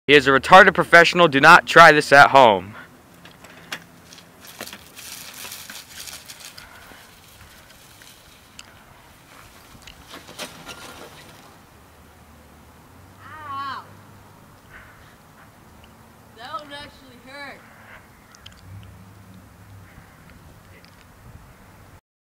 Bike Crash On Ice (Take 1)
My Friend Jesse and I were at Lions Park. We decided to screw around (*cough*cough*me,) For a fun little crash stunt, I decided to ride on the ice and make a slight turn to see what would happen. Well I slid. It was fun...with a touch of pain hahahaha jk I'm not emo.
     Take one of three.